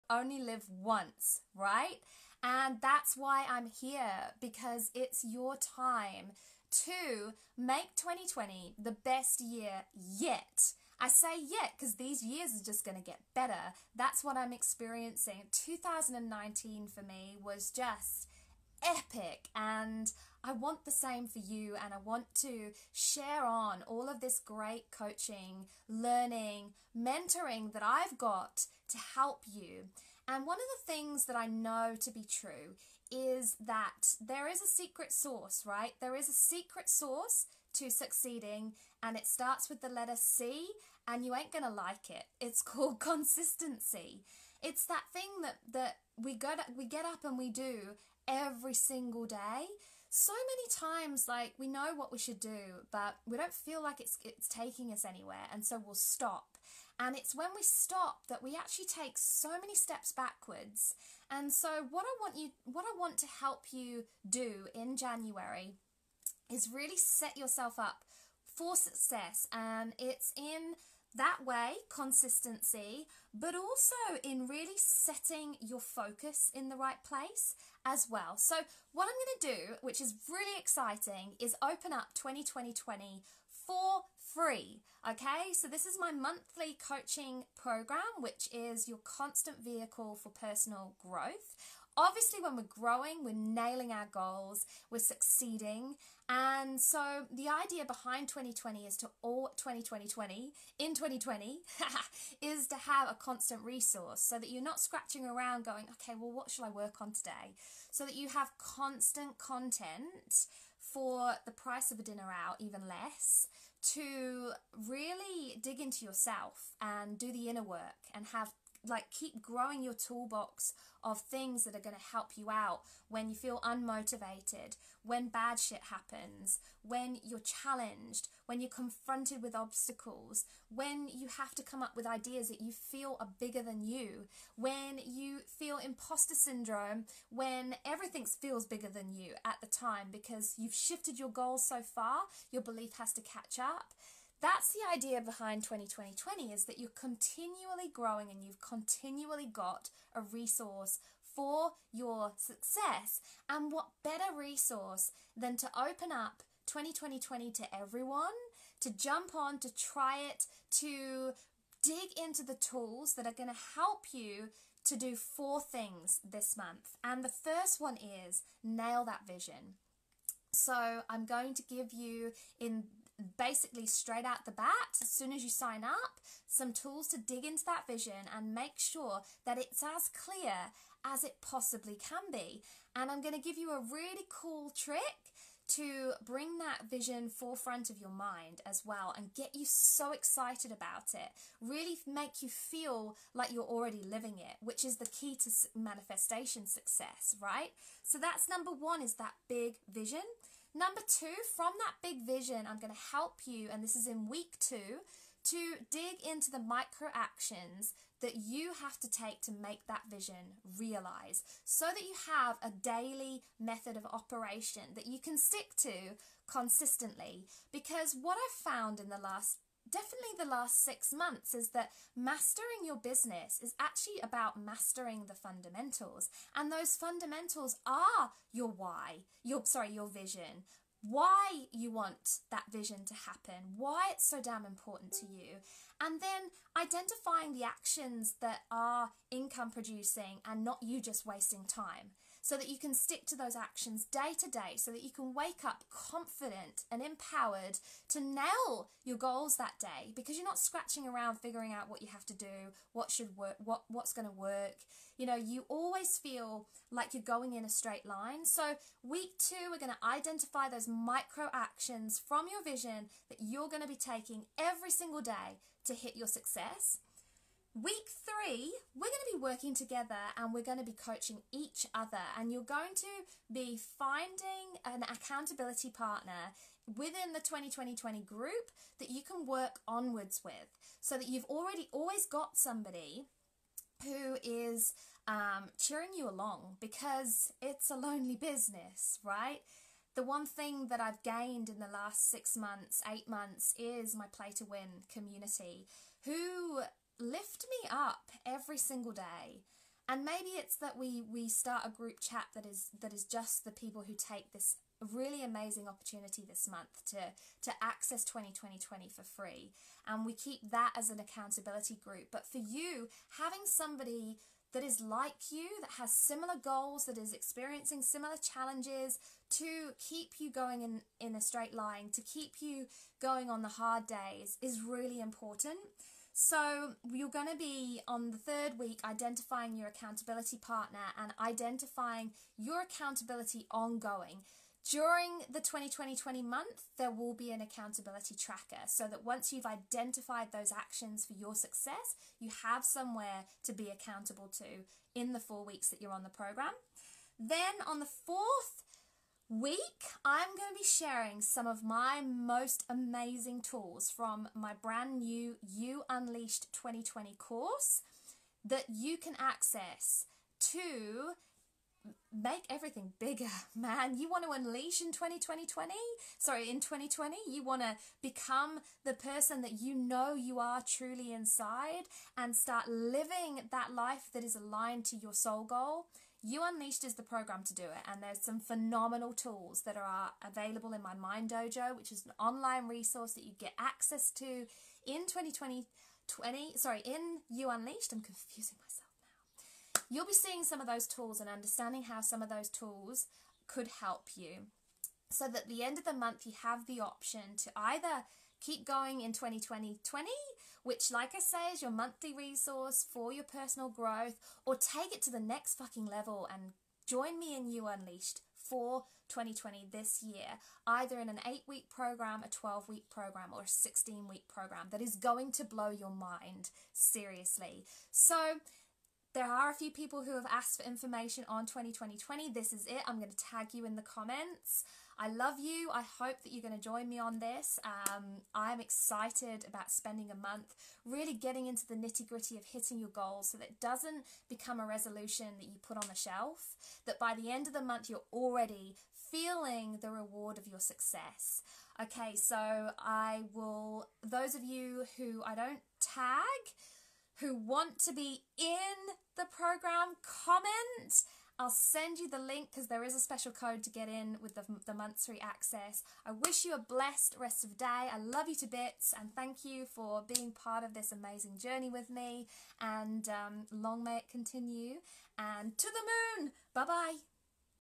How I'm going to help you ROCKET LAUNCH your new year goals!!!
Are you ready to make 2020 the year you start to live life on your terms and create the freedom-based lifestyle that is epically aligned to their Soul Goal?

I'm opening 20:20:20 up for free to new Members, on January 6, to help you kickstart this year on the right foot!

Across 4 weeks I'm going to help you set a Vision you will stick to and begin to action in a month of super-charged action!

Listen to how in the video!